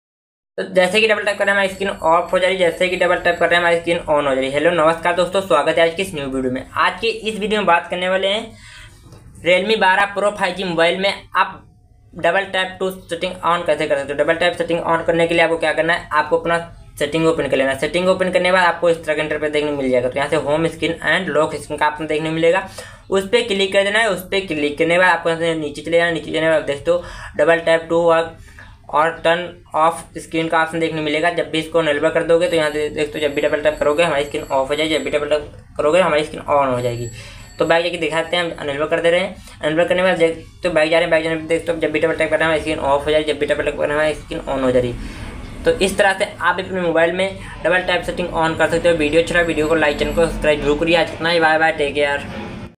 Realme 12 pro 5g mobile me double tap on settings kaise kare/how to set double tap on settings
Realme 12 pro 5g mobile me double tap on settings kaise kare/how to set double tap on settings realme 12 pro 5g mobile me

hello friends Maine is video me bataya hai ki realme 12 pro 5g mobile me double tap on off screen kaise kare is video ke madhyam se bataya hai